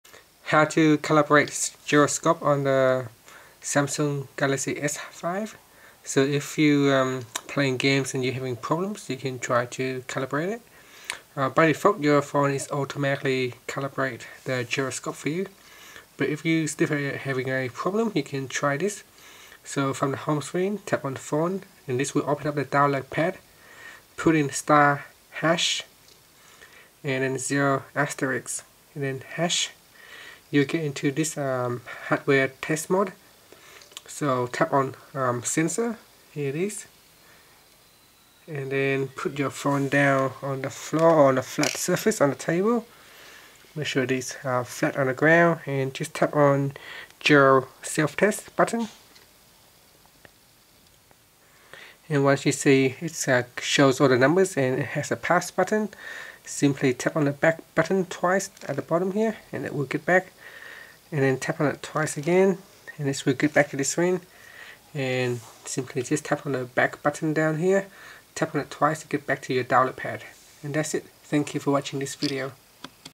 Samsung Galaxy S5: How to Calibrate Gyroscope Sensor With Selftest
Learn how you can calibrate the Gyroscope Sensor with the selftest feature on the Samsung Galaxy S5.